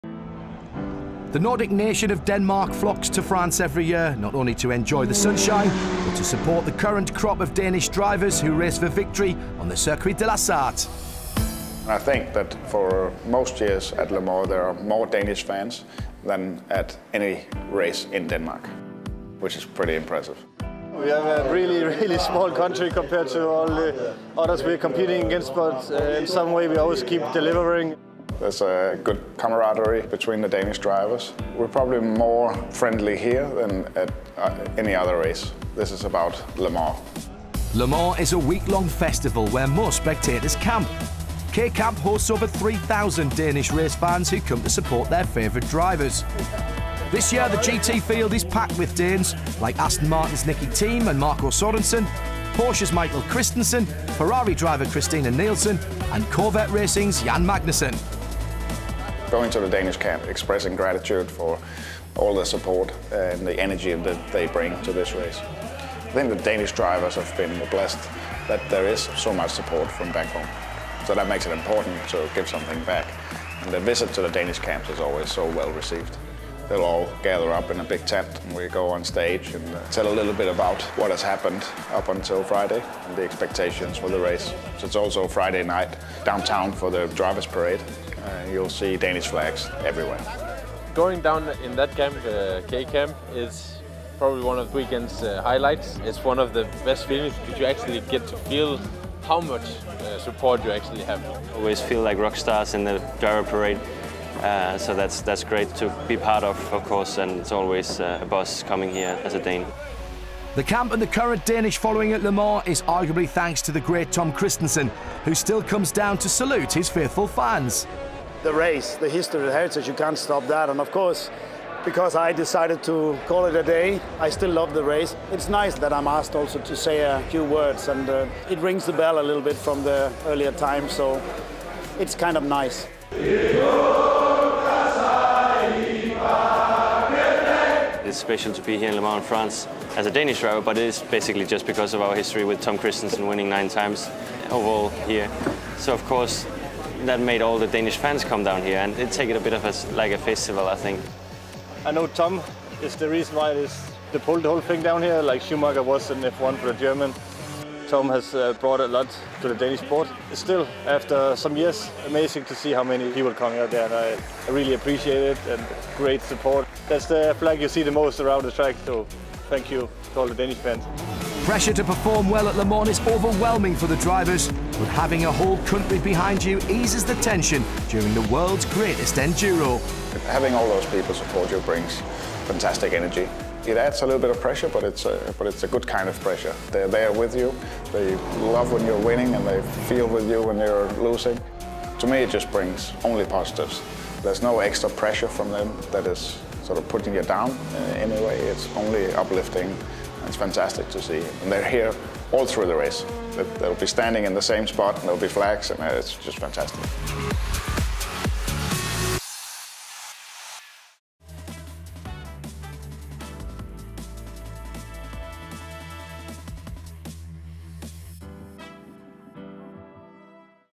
A Danish Celebration – Tom Kristensen, Jan Magnussen And More Meet The Fans At Le Mans | M1TG
Amongst the many thousands of motorsport fans that descend on Le Mans every summer, the Danes are arguably the most well-represented of them all. With a number of drivers representing the Nordic nation – including Jan Magnussen, Marco Sorensen, Christina Nielsen, Michael Christensen, Nicki Thiim and retired hero, 9-time winner Tom Kristensen – Denmark’s unofficial base at the Circuit de la Sarthe, otherwise known as ‘K-Camp’, is a home away from home for the traveling fans and drivers alike.